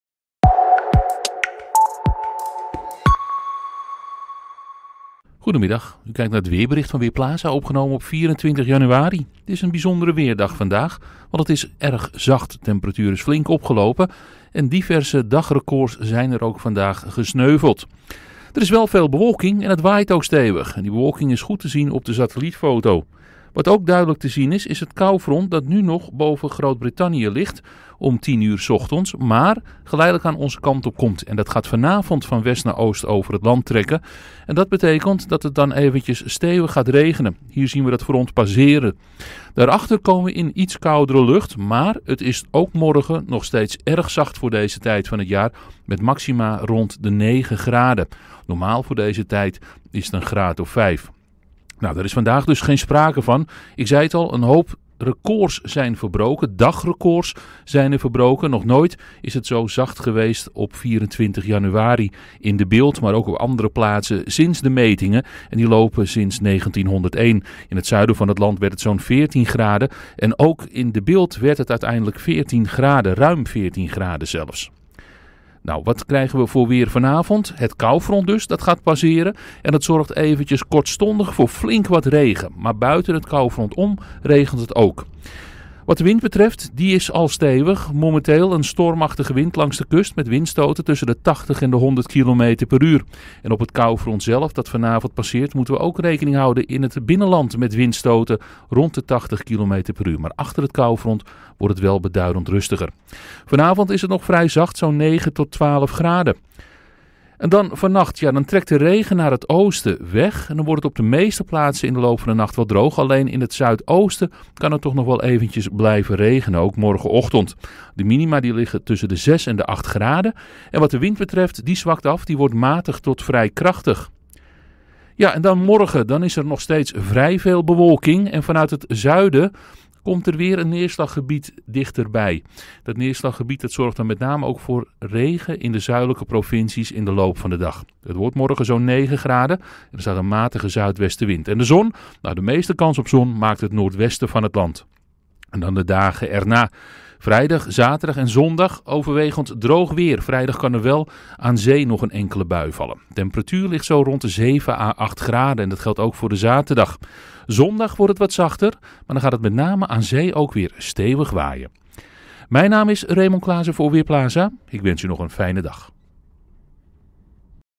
Weerbericht woensdagmiddag: Kalmer weer op komst
Het is vandaag bijzonder zacht en winderig met vanavond ook regen. Vanaf morgen wordt het weerbeeld rustiger.